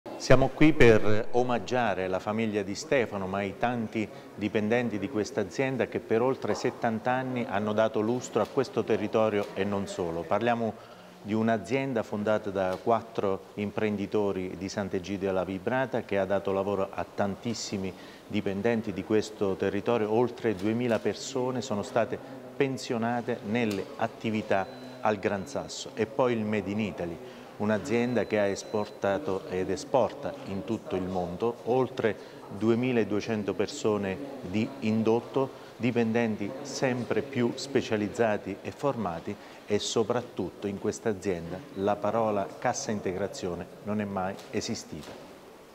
70 anni Maglificio Gran Sasso. Intervento del Consigliere Dino Pepe
Il consigliere regionale Dino Pepe, promotore del riconoscimento per i 70 anni del Maglificio Gran Sasso, interviene a margine della cerimonia che si è tenuta la mattina del 19 dicembre 2022 nella sede dell'azienda a Sant'Egidio alla Vibrata (TE).